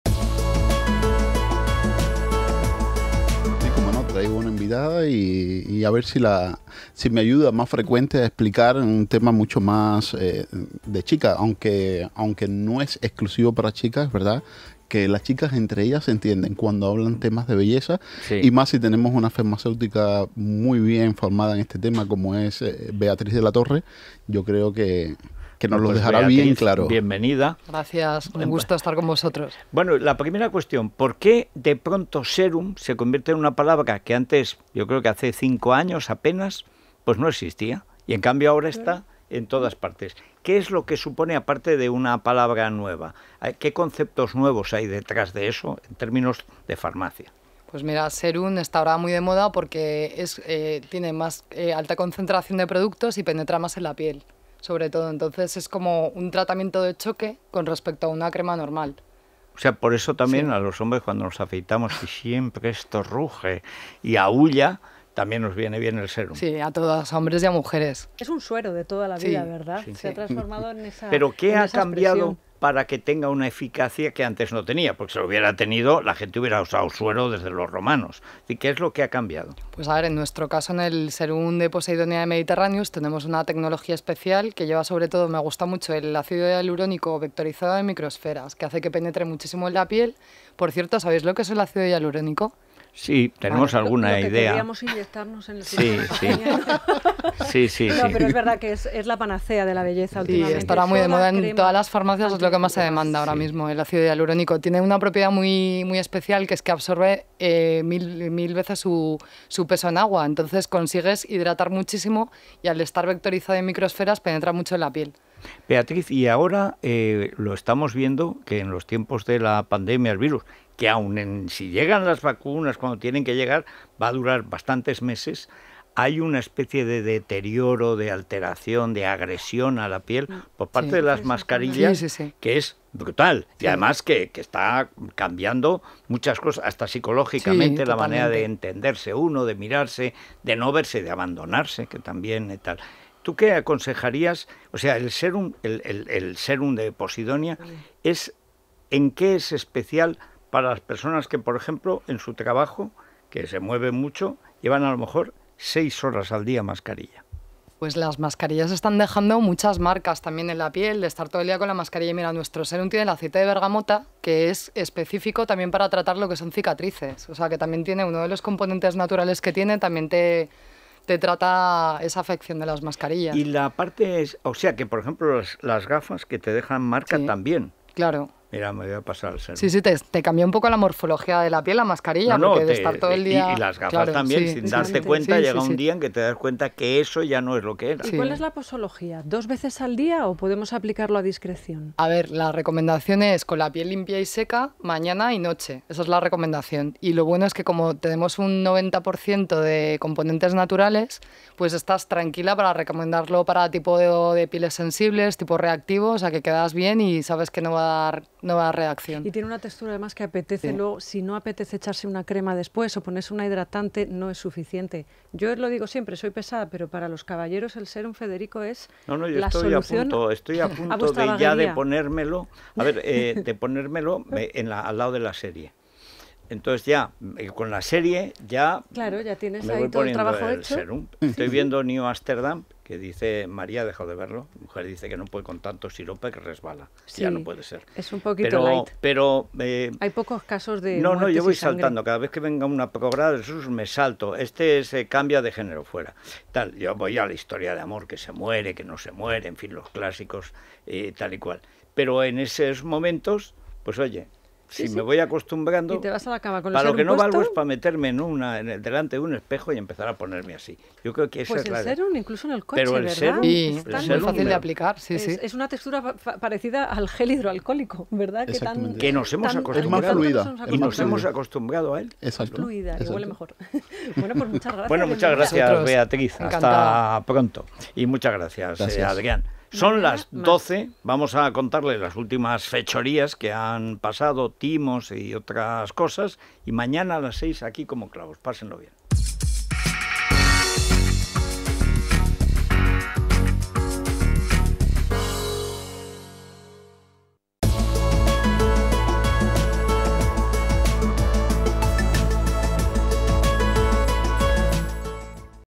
Mundo Natural: ¿Qué es es serum?
Federico e Isabel conversan de salud con Adrián González y Beatriz de la Torre.